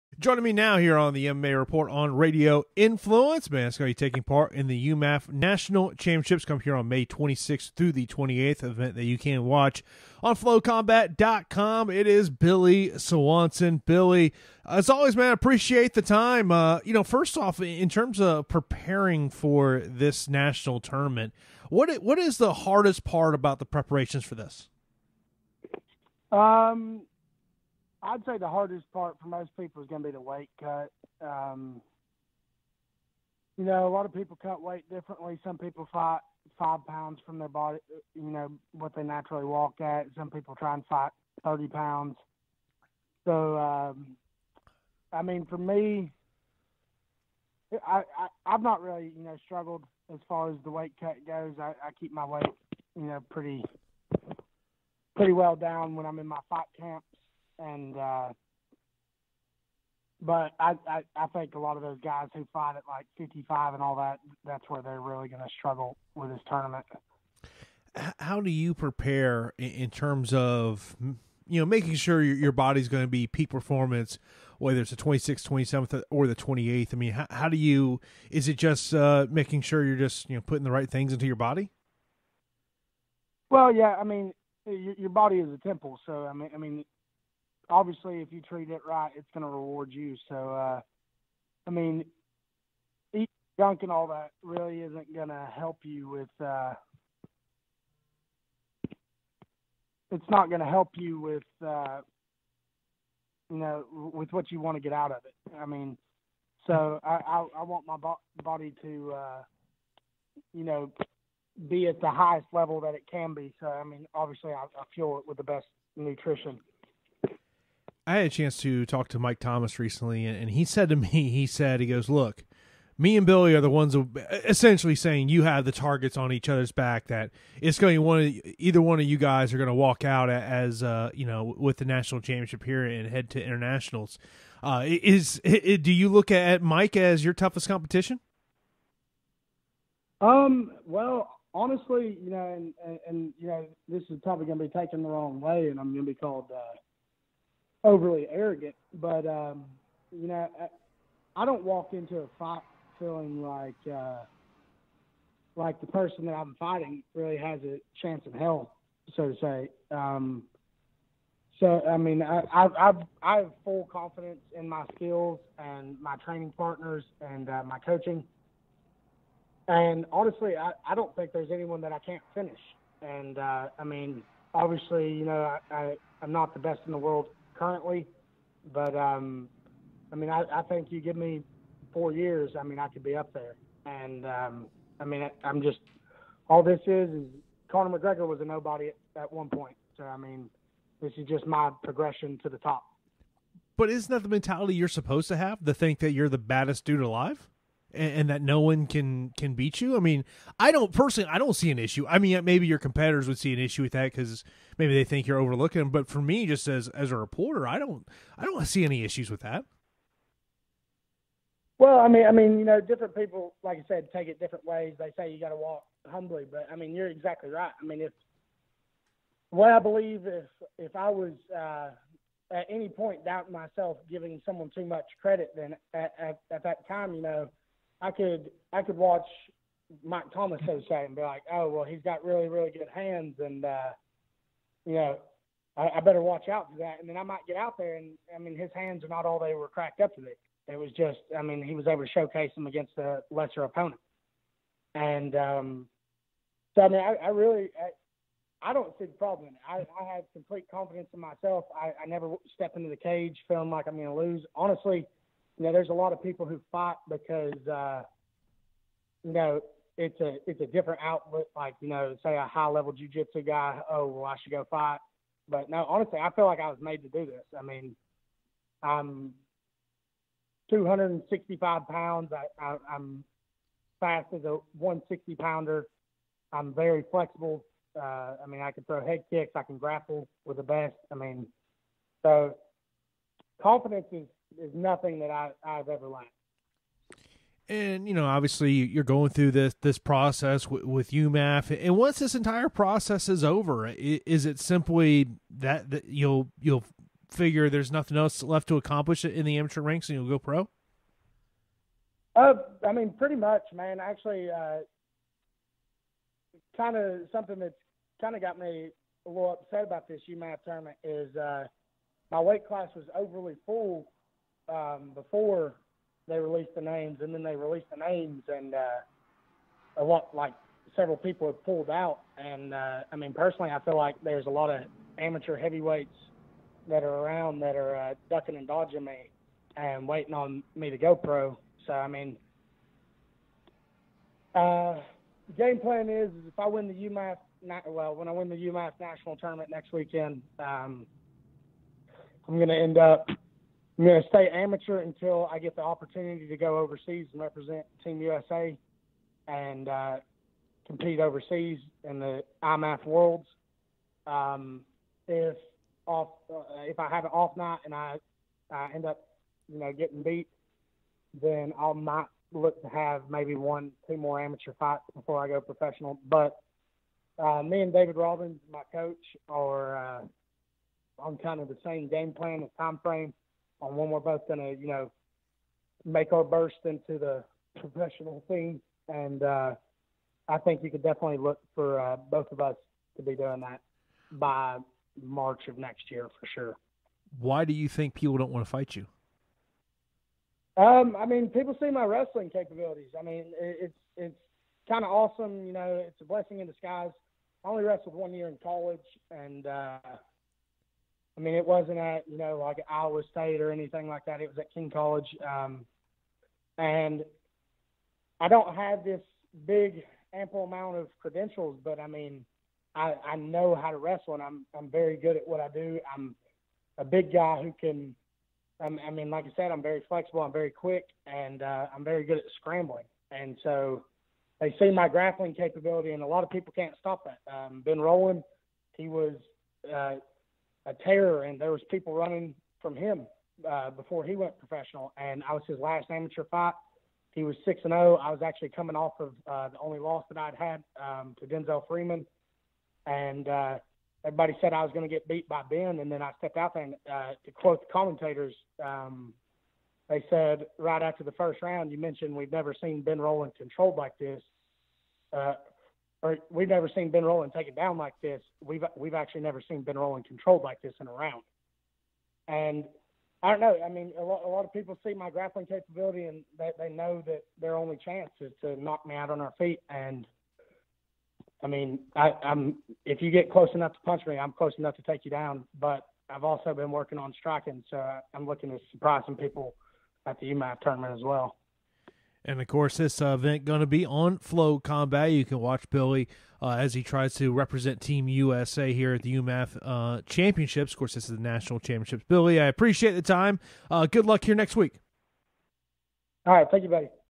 Heading into UMMAF, Billy Swanson says he doesn't think there's anyone he can't finish
Billy Swanson will be taking part in the UMMAF National Championship Tournament and he chats with Jason Floyd of The MMA Report about being part of the heavyweight tournament.